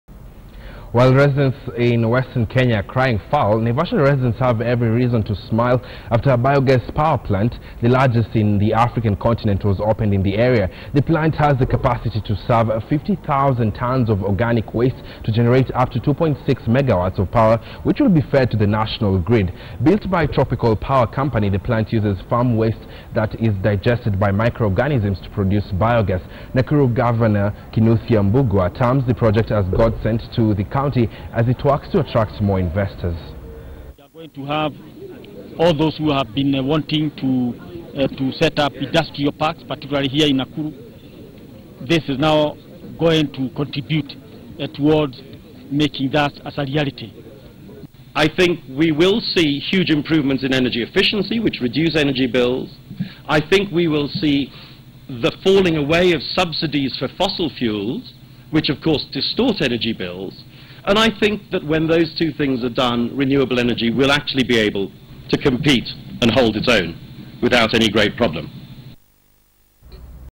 Largest biogas power plant in the African Continent opened in Naivasha
Naivasha residents have every reason to smile after a biogas power plant; the largest in the African continent was opened in the area. The plant has the capacity to recycle 50, 000 tonnes of organic waste to generate up to 2.6 megawatts of power which will be fed to the national grid. Built by tropical power company, the plant uses farm waste that is digested by micro-organisms to produce biogas. Nakuru governor Kinuthia Mbugua terms the project as a godsend to the county as it works to attract more investors.